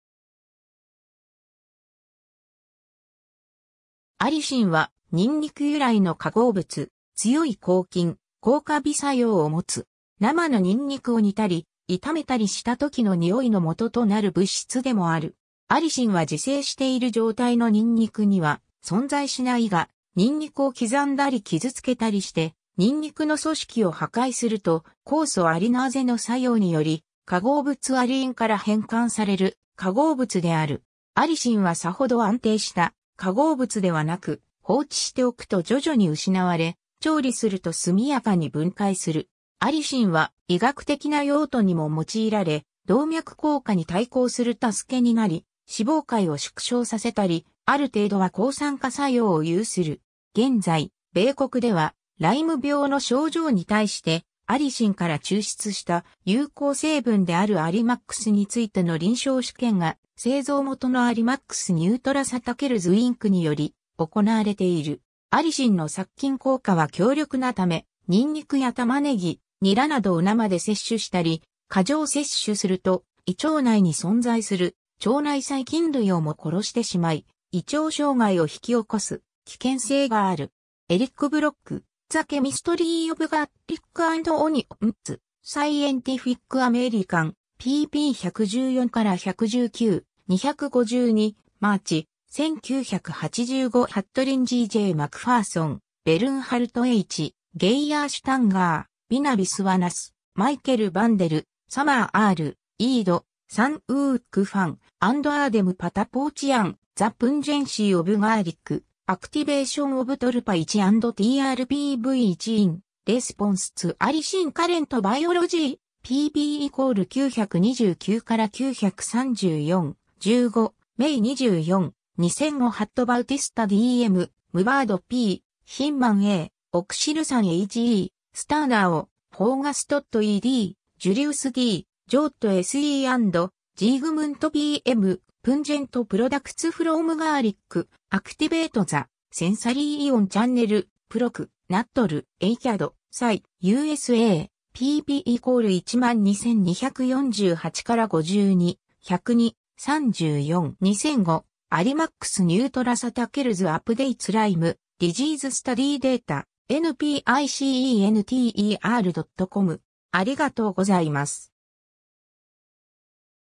アリシン
アリシン（allicin）は、ニンニク由来の化合物。
強い抗菌・抗カビ作用を持つ。
生のニンニクを煮たり炒めたりした時の臭いの元となる物質でもある。
 アリシンは自生している状態のニンニクには存在しないが、ニンニクを刻んだり傷つけたりしてニンニクの組織を破壊すると、酵素アリナーゼの作用により、化合物アリインから変換される化合物である。
 アリシンはさほど安定した化合物ではなく、放置しておくと徐々に失われ、調理すると速やかに分解する。
アリシンは医学的な用途にも用いられ、動脈硬化に対抗する助けになり、脂肪塊を縮小させたり、ある程度は抗酸化作用を有する。
 現在、米国ではライム病の症状に対してアリシンから抽出した有効成分であるAllimaxについての臨床試験が製造元のAllimax Neutraceuticals Incにより行われている。
 アリシンの殺菌効果は強力なため、ニンニクやタマネギ、ニラなどを生で摂取したり、過剰摂取すると、胃腸内に存在する腸内細菌類をも殺してしまい、胃腸障害を引き起こす危険性がある。